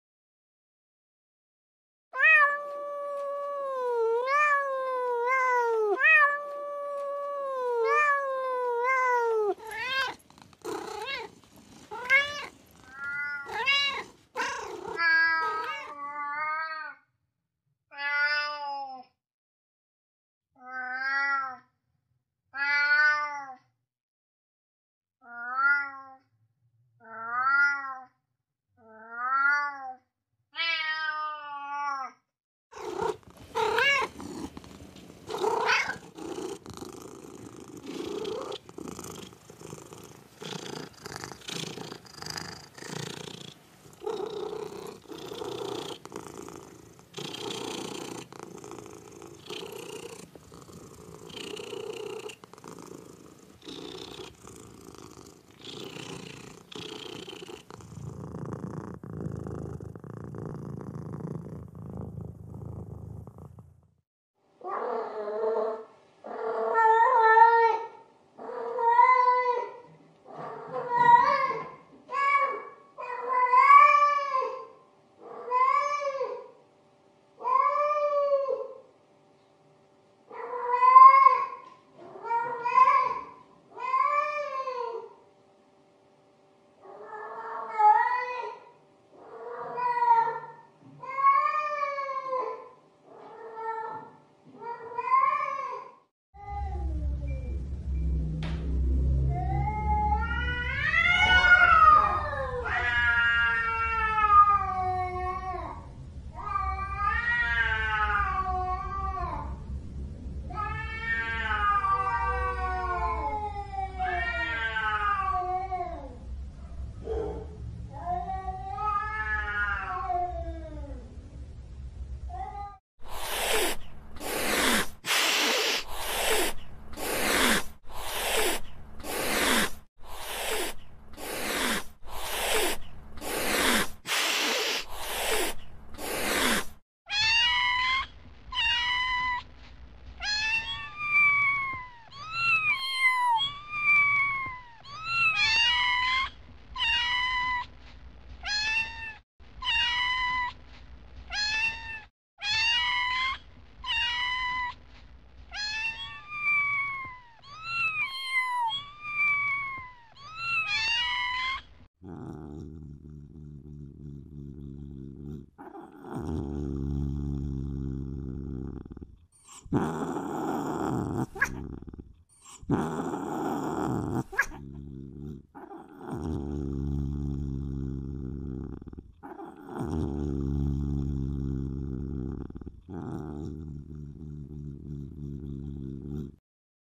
Cat sound effects|who likes cat?
Hello friends, Welcome to the amazing world of animal sounds.The cat (Felis catus) is a domestic species of small carnivorous mammal.It is the only domesticated species in the family Felidae and is commonly referred to as the domestic cat or house cat to distinguish it from the wild members of the family. A cat can either be a house cat, a farm cat, or a feral cat; the latter ranges freely and avoids human contact.Domestic cats are valued by humans for companionship and their ability to kill rodents. About 60 cat breeds are recognized by various cat registries.
Thanks
Bless up!😎